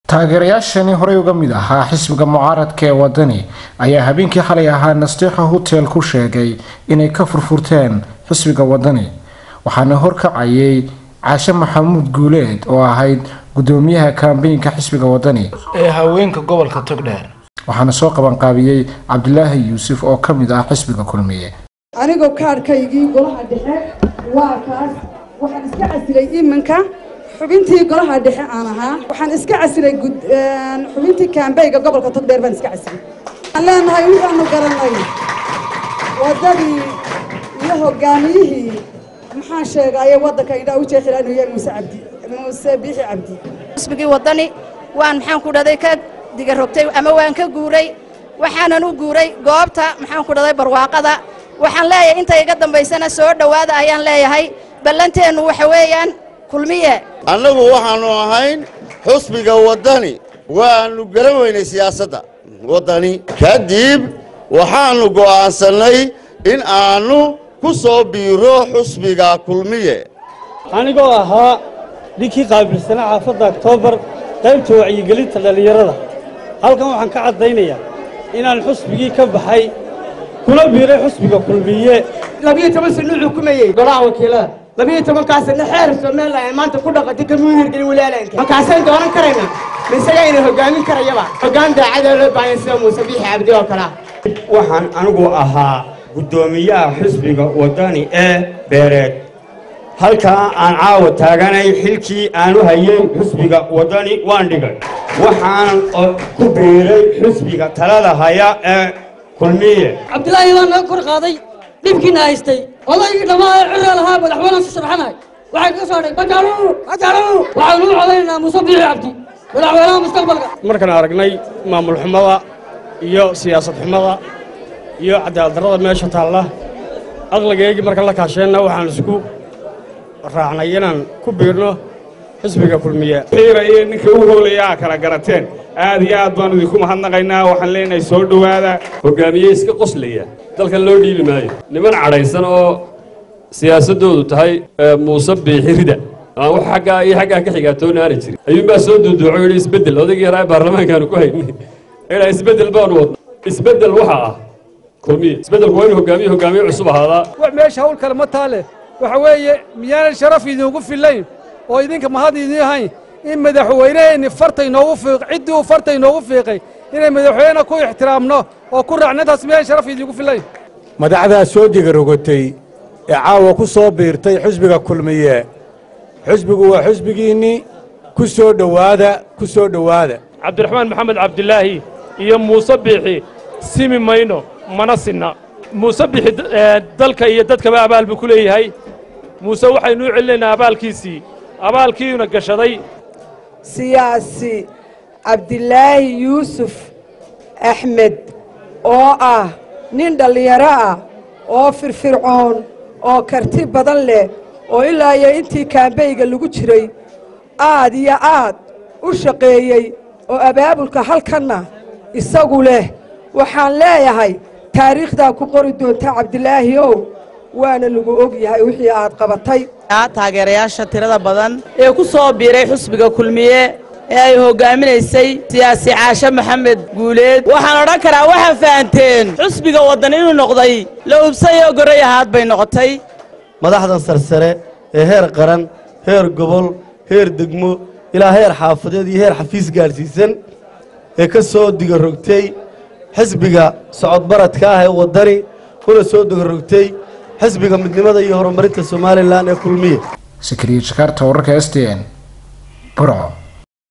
Daawo taagerayaal kusoo biiray xisbiga kulmiye magaalda burco
report by zakaria jakarta